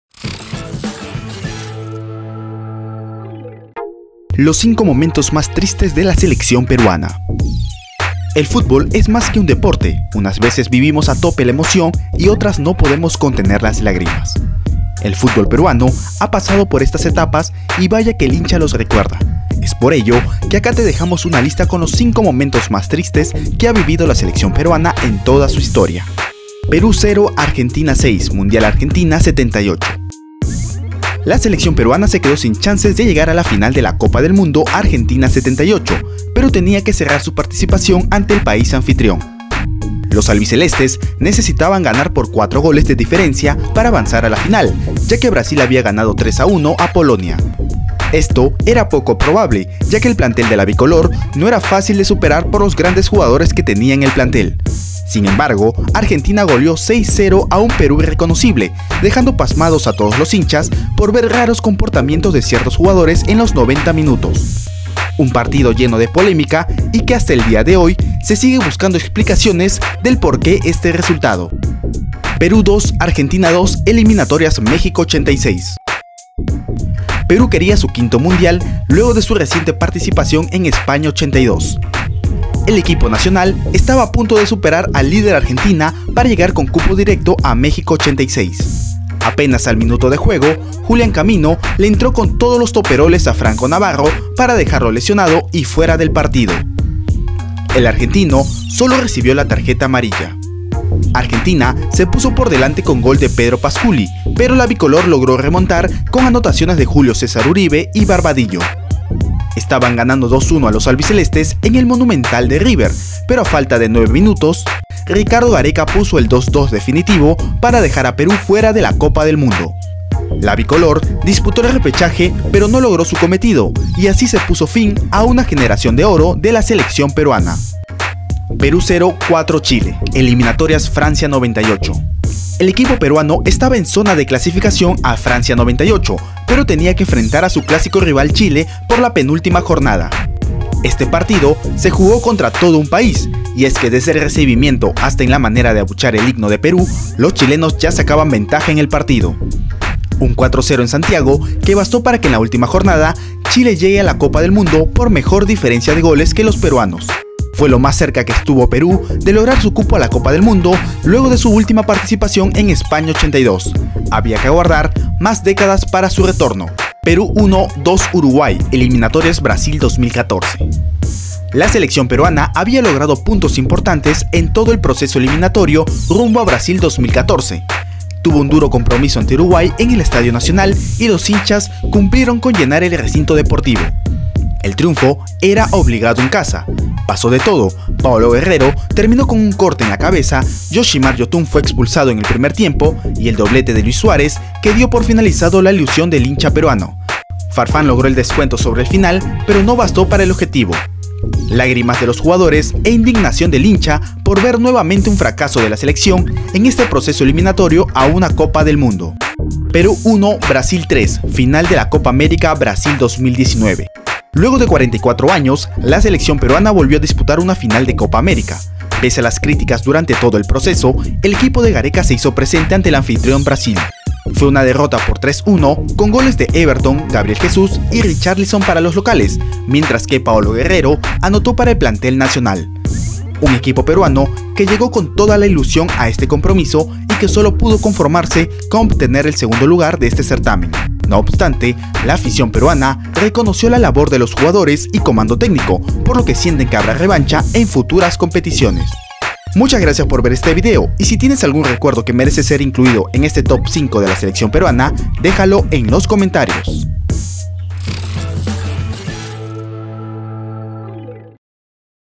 LOS 5 MOMENTOS MÁS TRISTES DE LA SELECCIÓN PERUANA EN TODA SU HISTORIA
El fútbol no solo es alegrías y eso lo sabe muy bien la Selección Peruana. La historia de la 'Bicolor' también tiene escenas grises que el hincha no puede olvidar. Es por ello, que te damos a conocer los 5 momentos más tristes de la Selección Peruana.